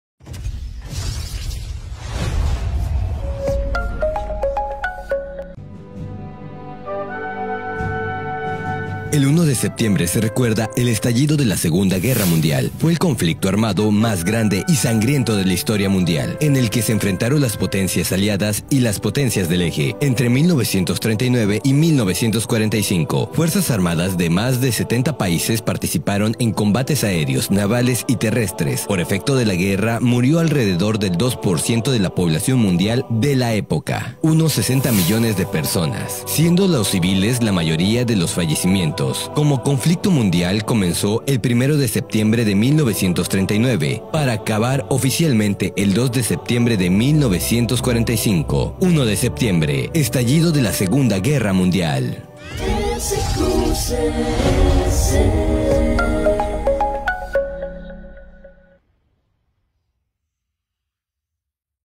SEGUNDA GUERRA MUNDIAL
El 1 de septiembre de 1939, Alemania invadió Polonia. Hitler pronosticó incorrectamente la respuesta occidental, ya que dos días después Francia y el Reino Unido le declararon la guerra dando inicio a la Segunda Guerra Mundial en Europa. El 17 de septiembre la Unión Soviética invadía Polonia desde el este.